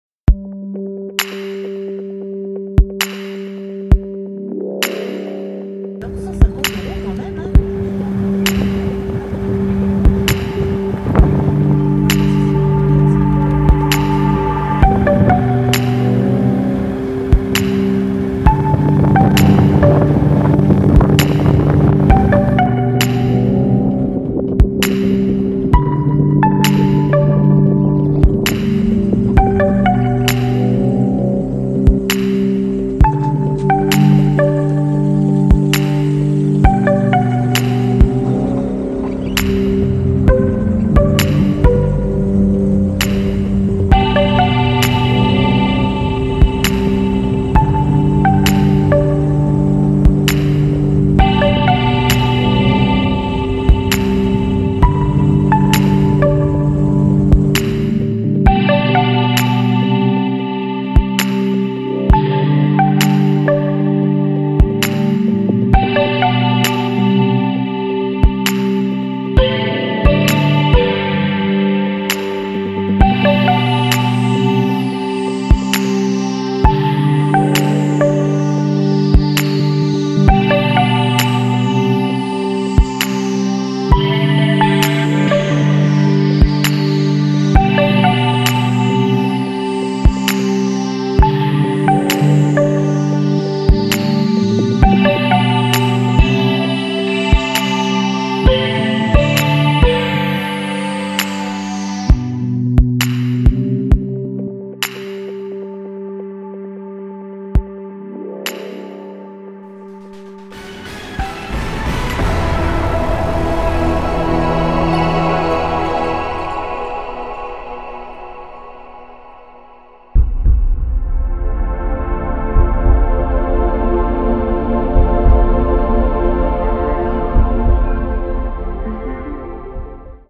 promenade en PAJERO
Plaine des Cafres, La Réunion en Mitsubishi Pajero ancienne génération.